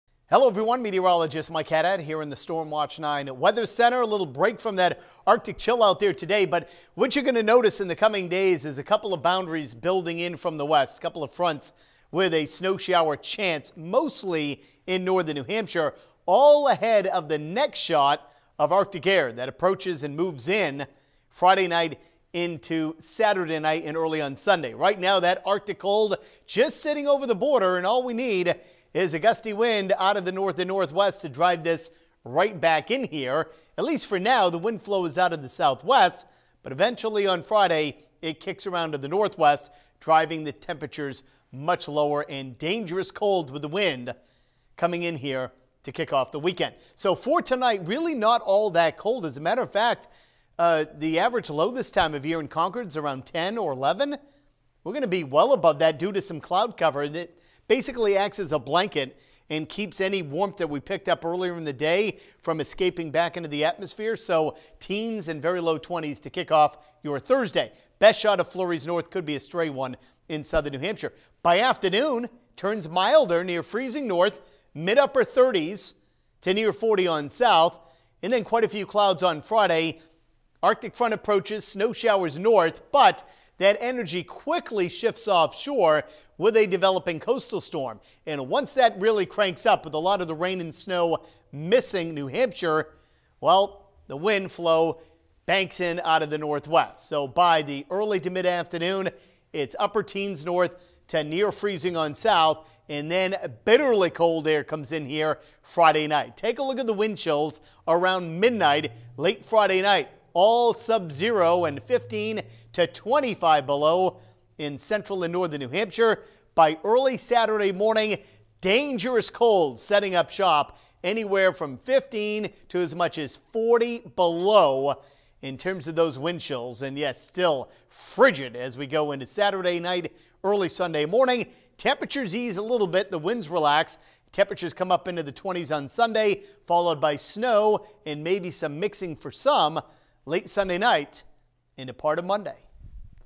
Update: Some snow shower chances before more cold air
Meteorologist Mike Haddad says there will be a couple snow shower chances ahead.